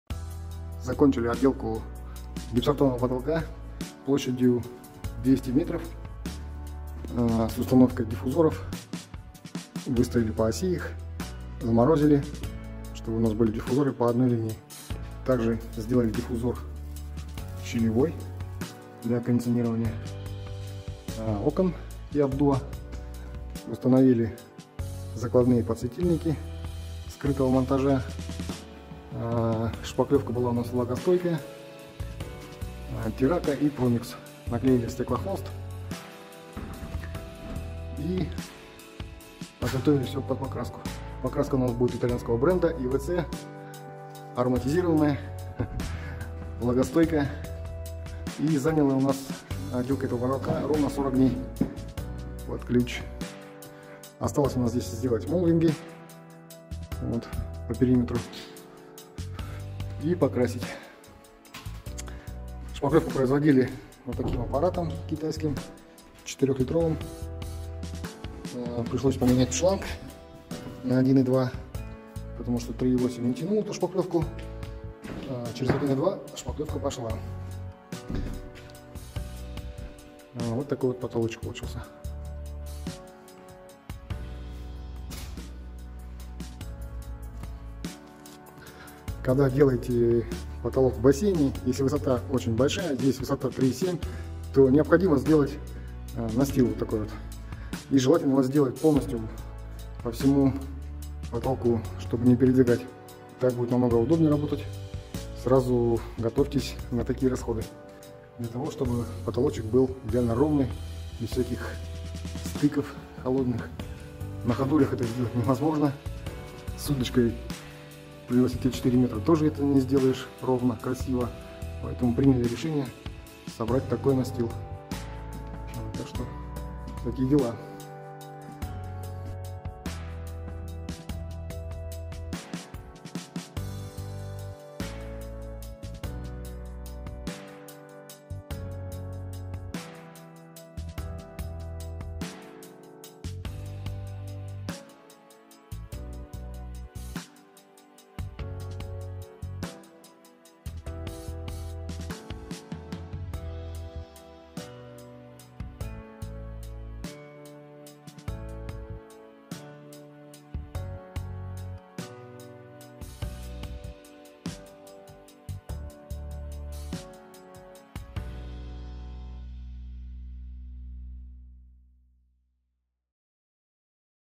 Шпаклёвка потолка в бассейне
Закончили шпаклевку потолка под покраску в помещении бассейна на высоте 3.7-3.9 метра.

Так как считается что это помещение с повышенной влажности применили влагостойкую шпаклёвку и гипсокартон в идеале потолок сделать из цементных листов Кнауф.

Монтаж вентиляции производила другая компания а вот финишную подготовку уже наша команда.

Установку закладных под скрытый светильник делаем на черновой отделке, а потом после покраски уже в ввинчивается сам светодиод.